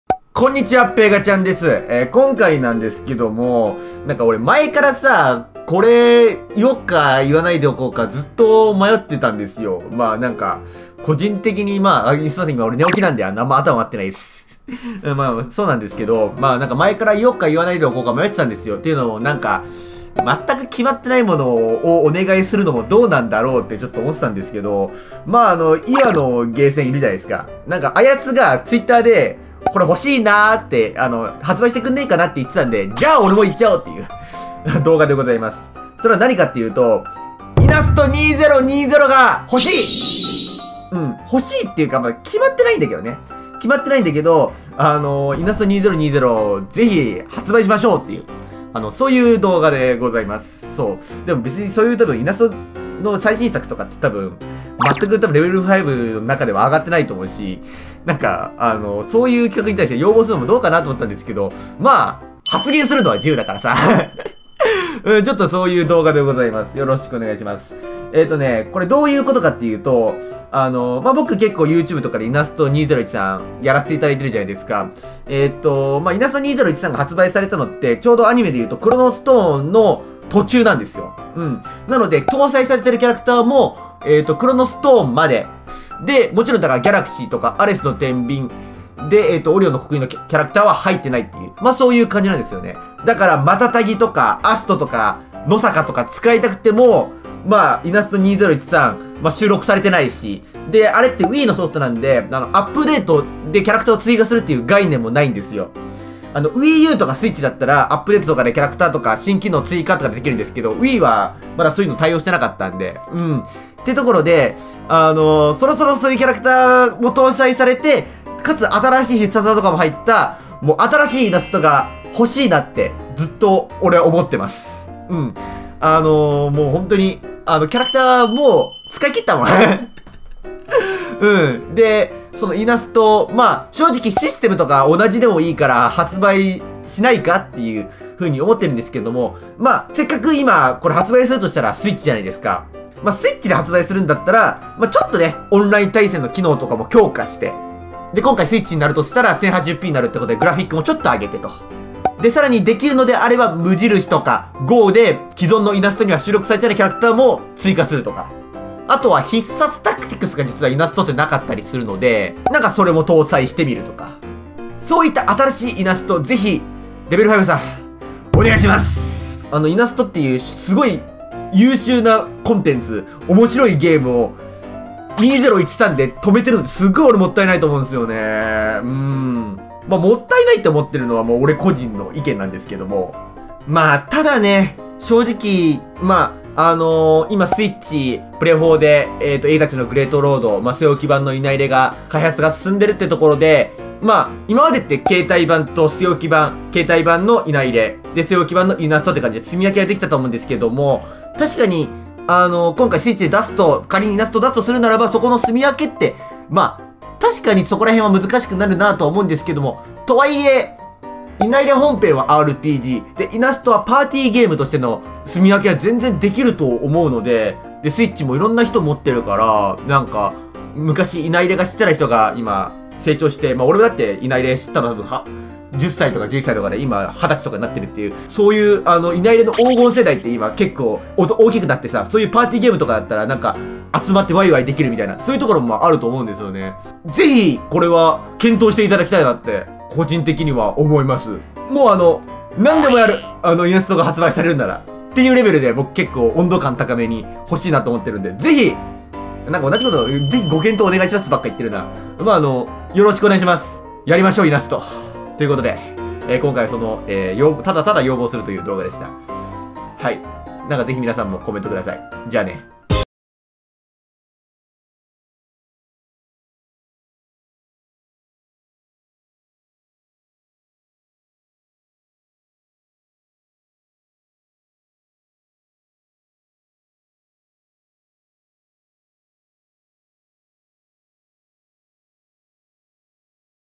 【熱望】イナズマイレブンストライカーズ2020の発売をただただお願いする動画
お手紙、プレゼントはこちら→ 【〒106-6137 東京都港区六本木6-10-1 六本木ヒルズ森タワー37F（South） テレペガ宛】

◆本動画内の音楽については、STAR MUSICの許可を頂いております。